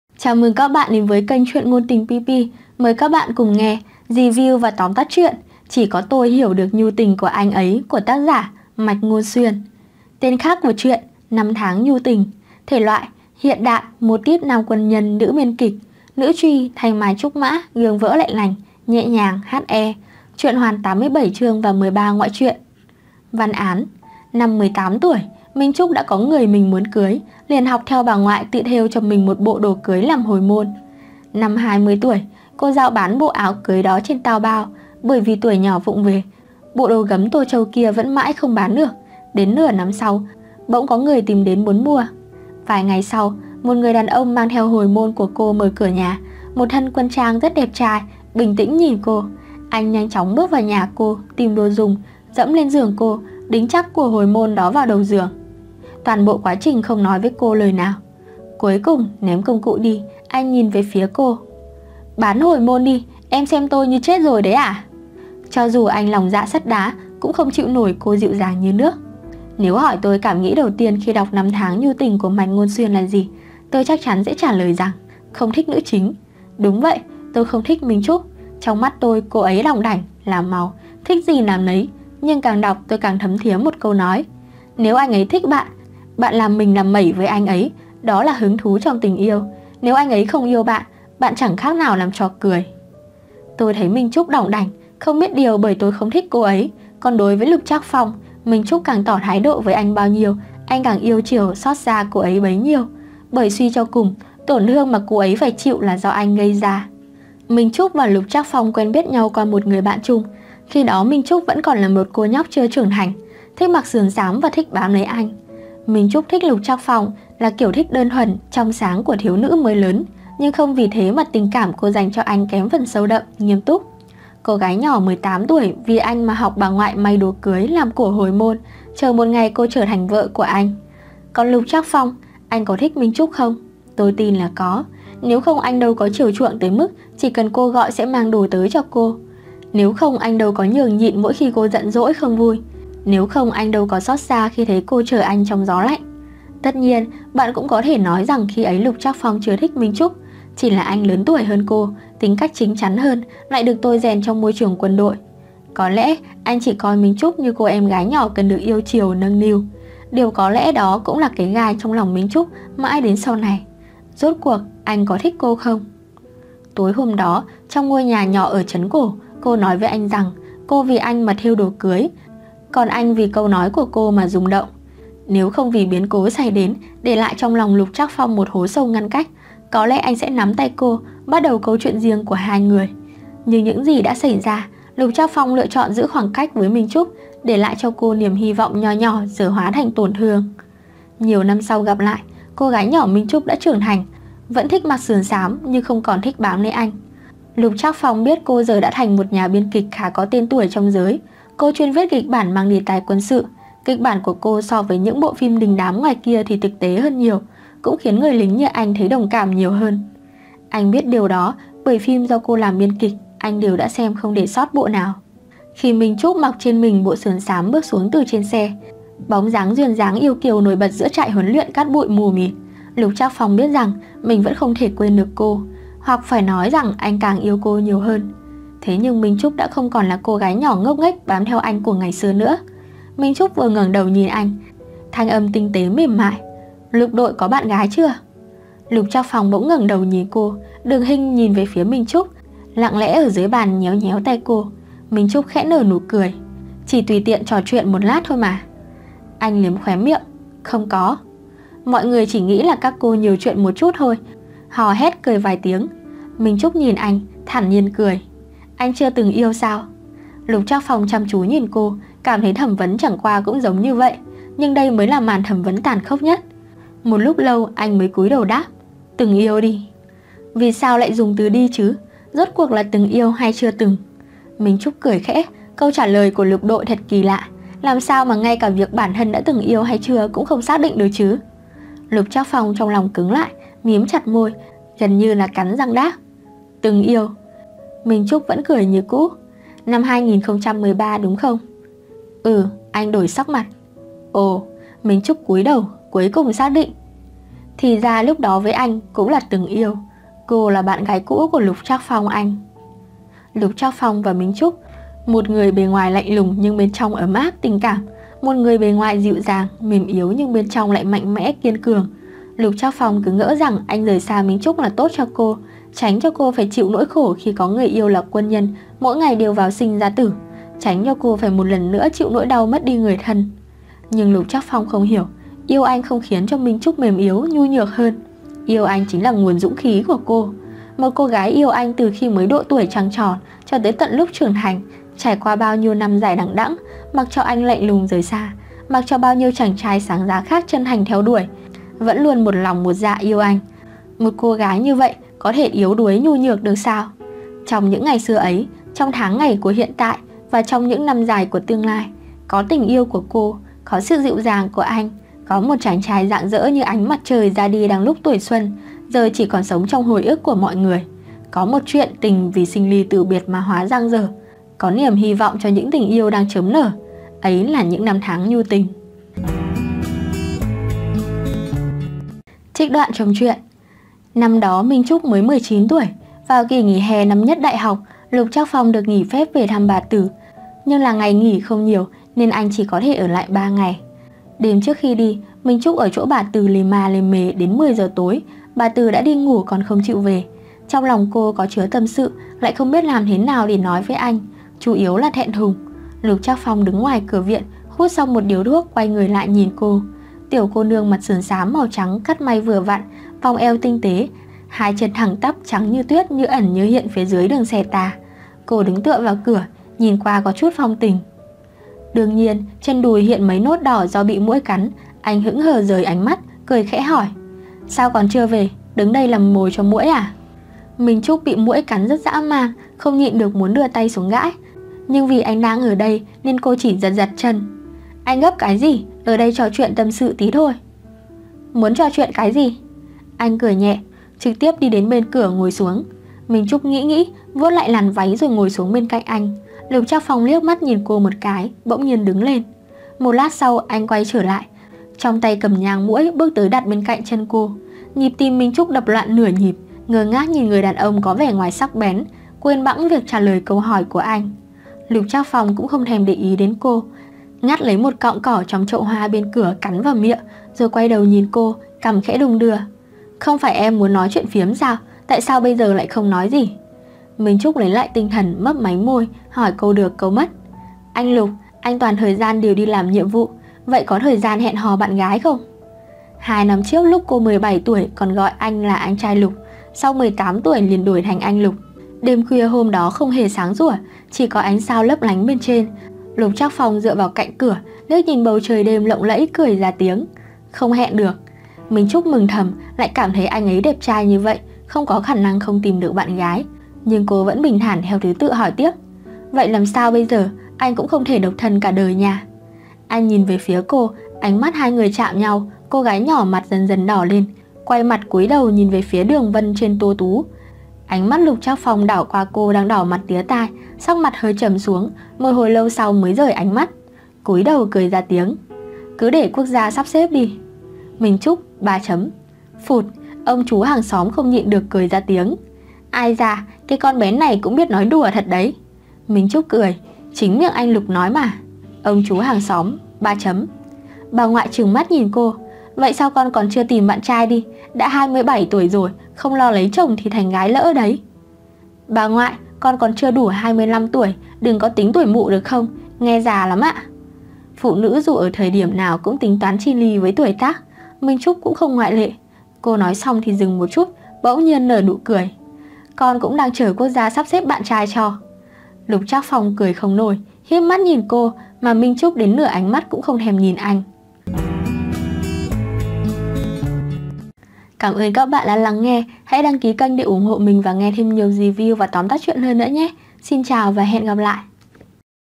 CHỈ CÓ TÔI HIỂU ĐƯỢC NHU TÌNH CỦA ANH ẤY - Mạch Ngôn Xuyên | Review Và Tóm Tắt | Truyện Ngôn Tình
Chào mừng bạn đến với kênh Truyện Ngôn Tình PiPi có GIỌNG ĐỌC THẬT truyền cảm. Mời các bạn cùng nghe Review Và Tóm Tắt Truyện CHỈ CÓ TÔI HIỂU ĐƯỢC NHU TÌNH CỦA ANH ẤY của tác giả Mạch Ngôn Xuyên

- Tên truyện: Chỉ Có Tôi Hiểu Được Nhu Tình Của Anh Ấy
- Tác giả: Mạch Ngôn Xuyên
- Tên khác: Năm Tháng Nhu Tình
- Thể loại: Hiện đại, motif nam quân nhân x nữ biên kịch, nữ truy, thanh mai trúc mã, gương vỡ lại lành, nhẹ nhàng, HE
- Tình trạng: hoàn 87 chương + 13 ngoại truyện.
- Review Nhã Phi
Giọng đọc PiPi
&.o
Minh Chúc năm 18 tuổi đã có người trong lòng, liền theo bà ngoại học thêu cho mình một bộ đồ cưới làm của hồi môn.
Năm 25 tuổi, cô đem đồ cưới của chính mình rao bán trên Taobao, bởi vì lúc trước tay nghề vụng về, bộ đồ gấm Tô Châu mãi vẫn không bán được. Nửa năm sau, đột nhiên có người tới tận cửa tìm mua.